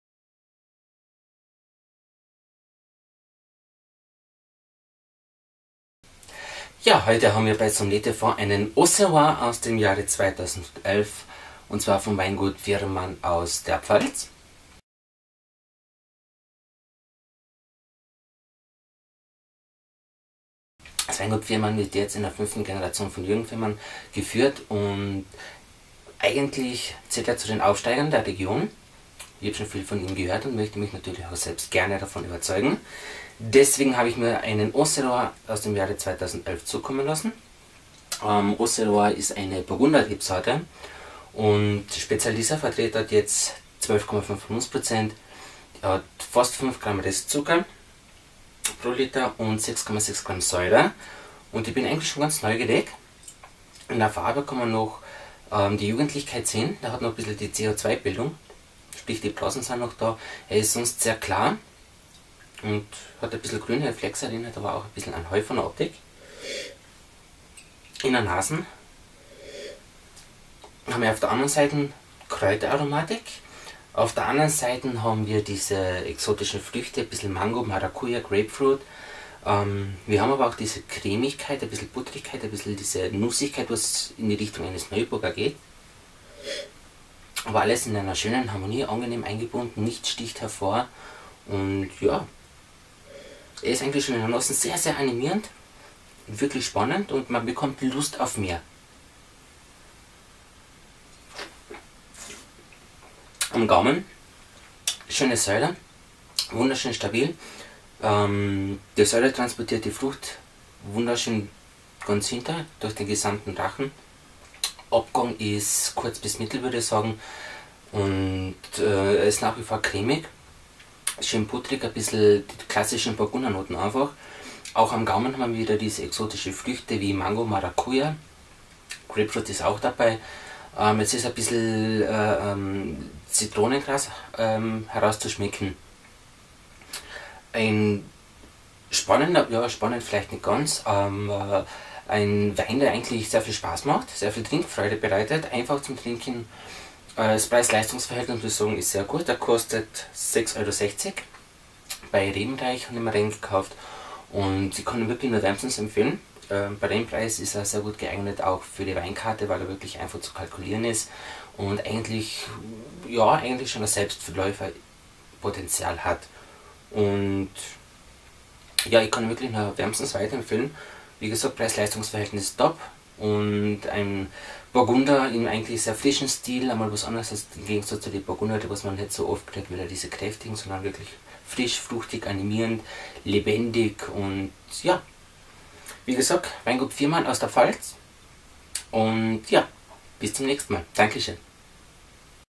Auxerrois 2011 vom Weingut Pfirmann aus der Pfalz | Sommelier TV - by Peter Ladinig
Auxerrois ist eine weiße Rebsorte. Der Name ist wahrscheinlich von dem der französischen Grafschaft Auxerre abgeleitet, aus deren Umgebung sie stammt. Die Auxerrois-Rebe gehört zur Gruppe der Burgunder und ist eine natürliche Kreuzung zwischen Pinot und Gouais Blanc (Weißer Heunisch).